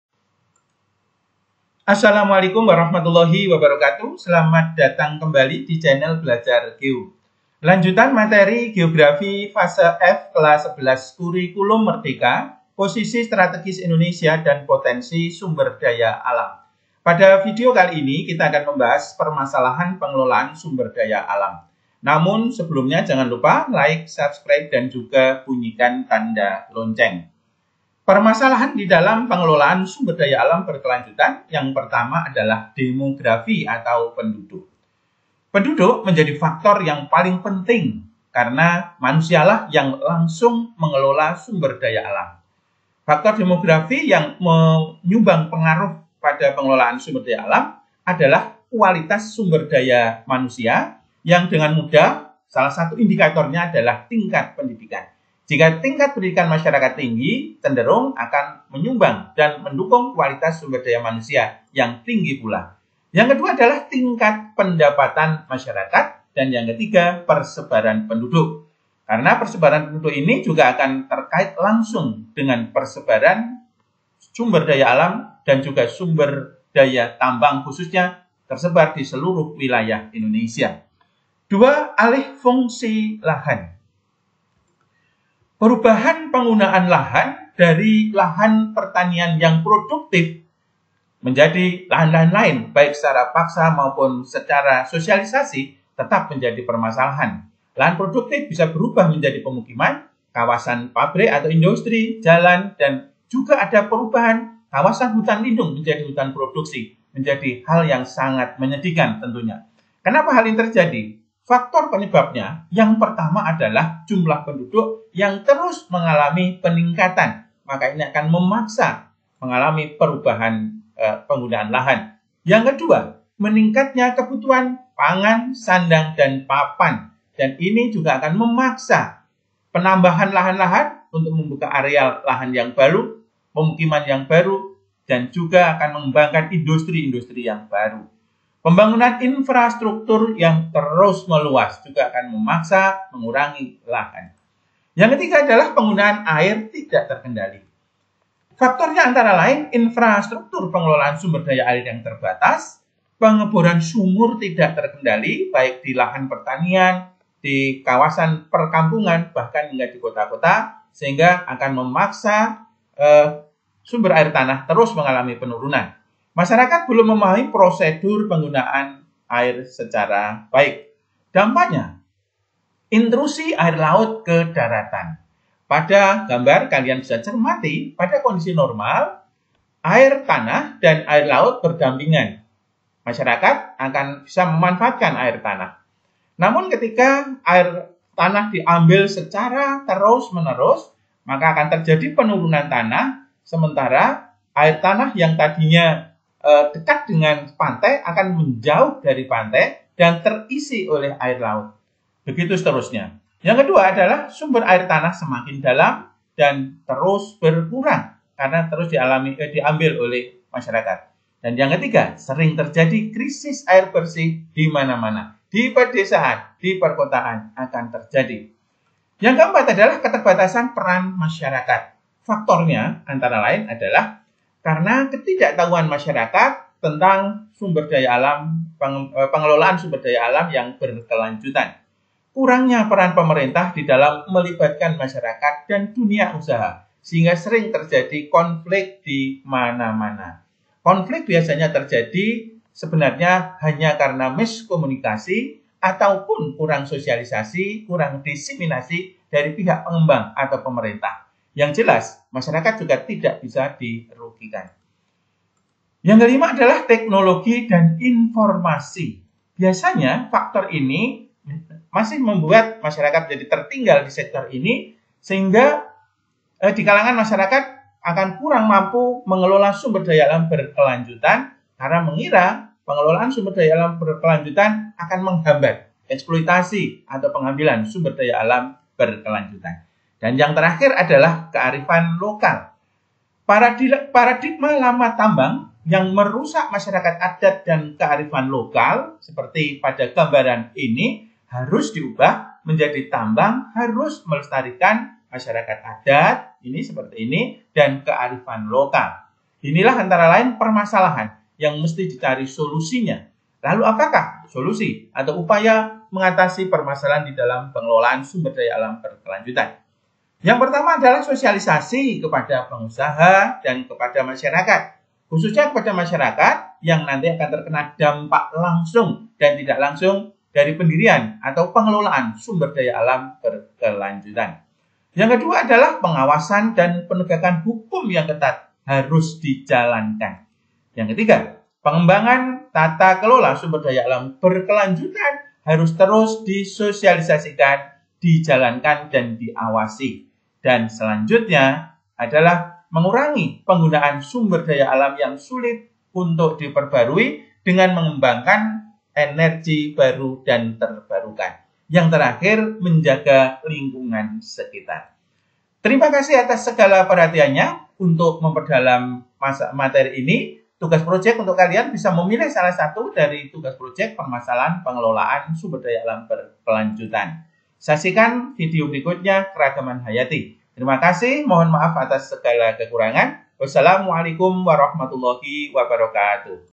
Permasalahan Pengelolaan SDA kumer klas XI
Selamat datang di Channel Belajar Geo, Vidio berikut membahas Permasalahan Pengelolaan SDA Berkelanjutan (bag 2). Saksikan sampai selesai supaya anda paham. Semoga bermanfaat.

Lembaga Yang Berperan Penanggulangan Bencana, Link : https;//youtu.be/2QhszWfa5bk